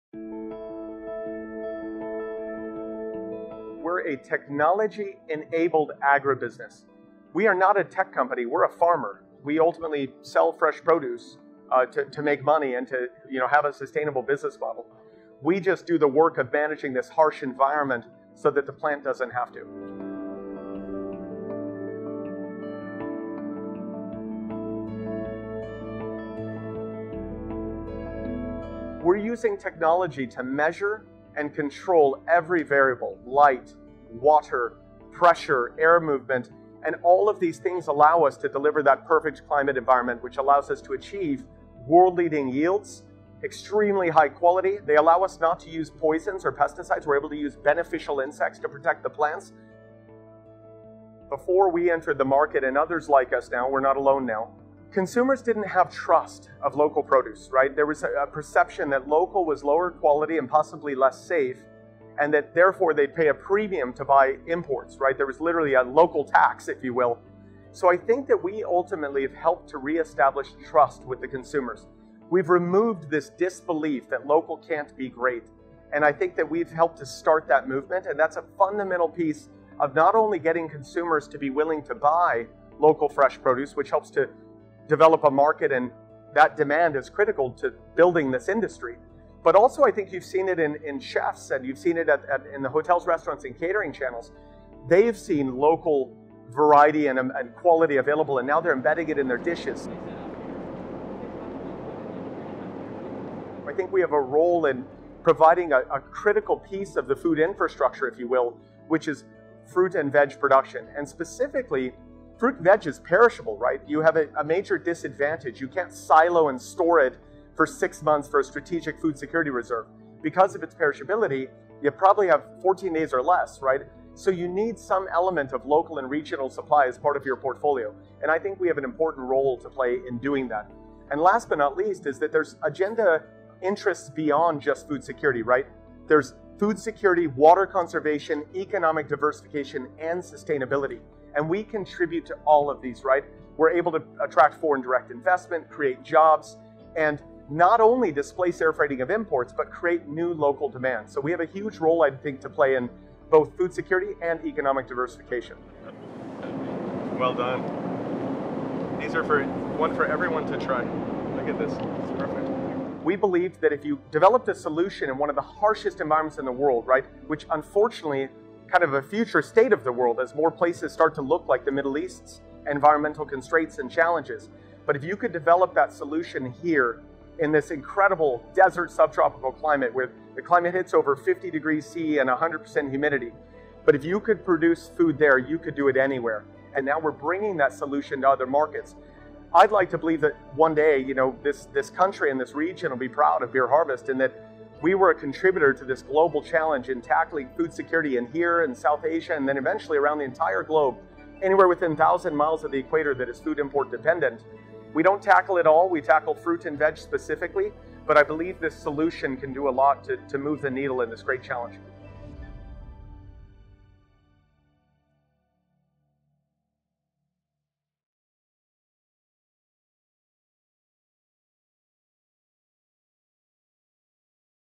How One Company Uses Technology to Grow Tomatoes in the Desert
Sky Kurtz, Co-founder and CEO of Pure Harvest Smart Farms, believes in failing small to win big. At his pilot farm in the desert outside Abu Dhabi, where temperatures regularly top 45 degrees Celsius and camels graze outside the gates, Kurtz and his team trial new crops and technologies before rolling out production across their larger facilities. The agri-tech solutions that Pure Harvest is developing have the potential to change farming in climate-challenged areas as we know it.